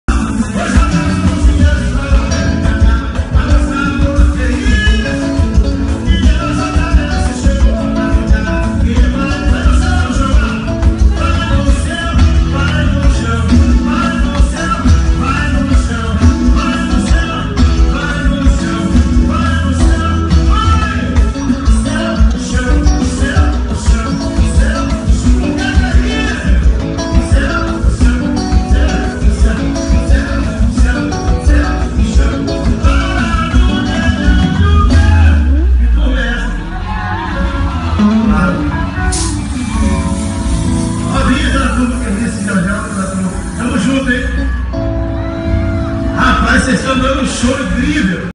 Jean Santana no bloco Lá Vem Eles
Cantor feirense agitou bloco travestido onde mulheres se fantasiaram de homem na Micareta de Feira 2017.